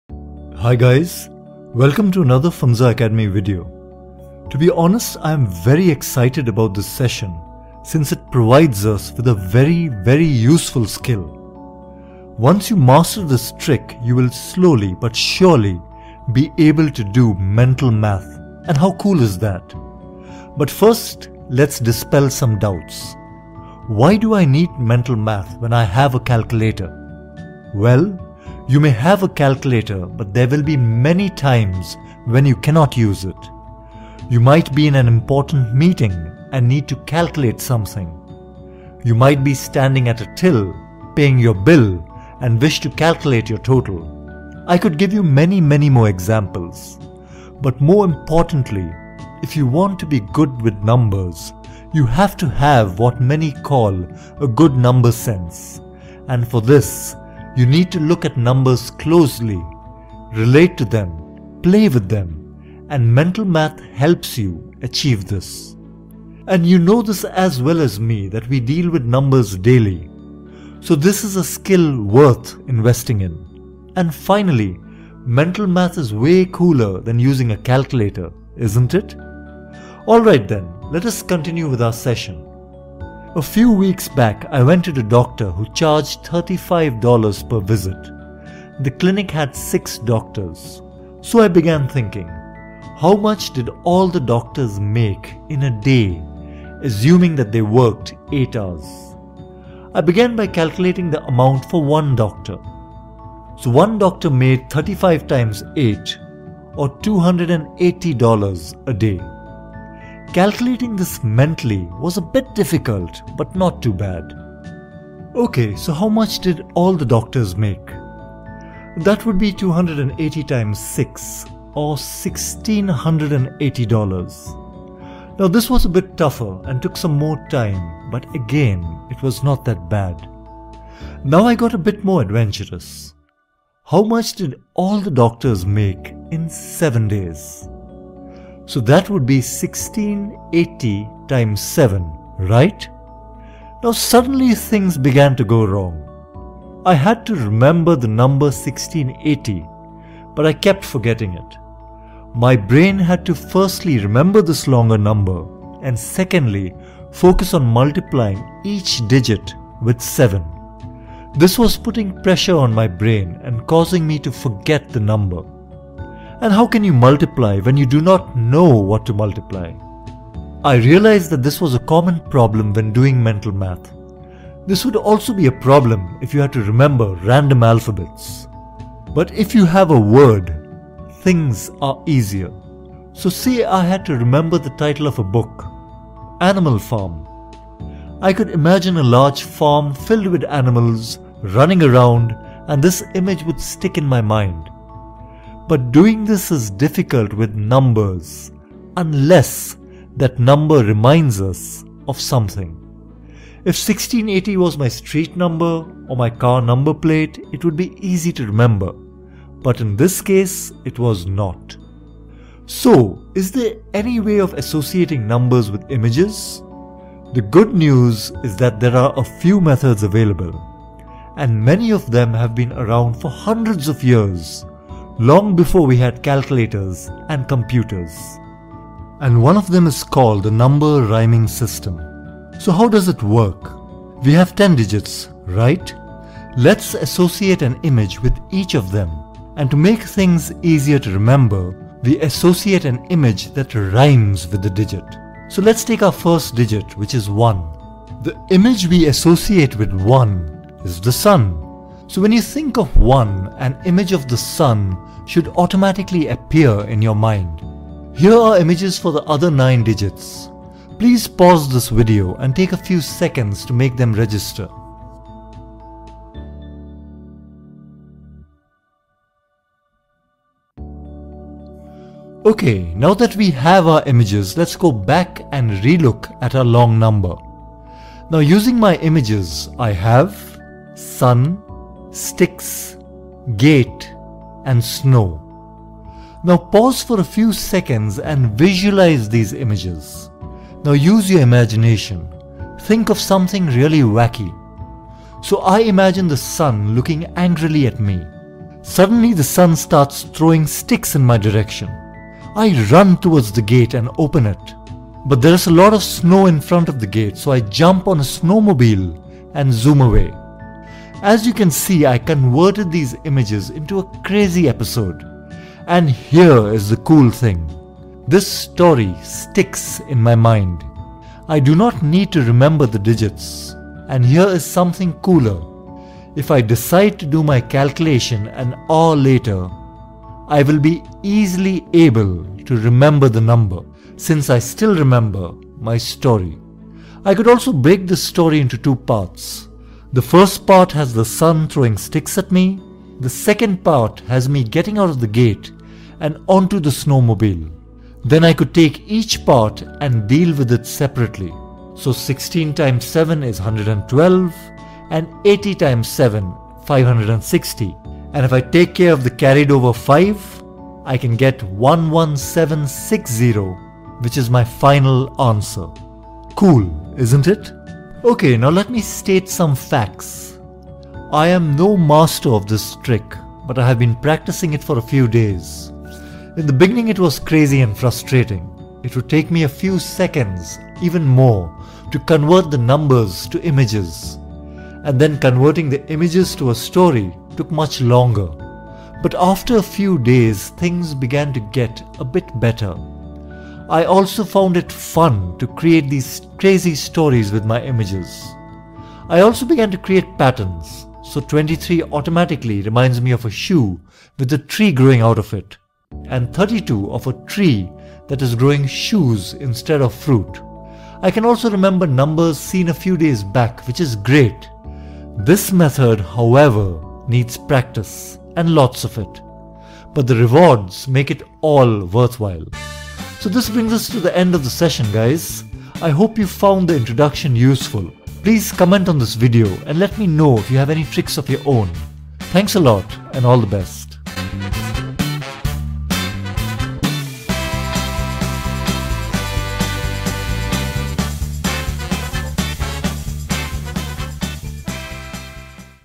How to multiply in your Head : Mental Math Tricks
How to multiply in your Head : Mental Math Tricks An engaging video that will provide you tips and strategies to do Mental Math.
This will help you acquire  a Number Sense, critical for developing good math skills.Tips on how to multiply faster than ever! Suitable for young learners and professionals. Enjoy.

Overview:
Why do I need mental math when I have a calculator ?
Well, you may have a calculator but there will be many times when you cannot use it.
You might be in an important meeting and need to calculate something
You may be standing at a till, paying your bill and wish to check your total.
I could give you many more examples.
But more importantly, if you want to be good with numbers, you have to have what many call a good "number sense".
And for this, you need to look at them closely, relate to them, play with them.
And mental math helps you achieve this.
And you know this as well as me that we deal with numbers DAILY.
So this is a skill worth investing in.
And finally, mental math is way cooler than using a calculator, is'nt it?

Tools Used:
Microphone : Samson Meteor
Voice Editor : Audacity
Animation : Sparkol Video scribe
Video editor + SFX : iMovie

Individuals involved in Video:
Sanjay Noronha, Preeti Kaul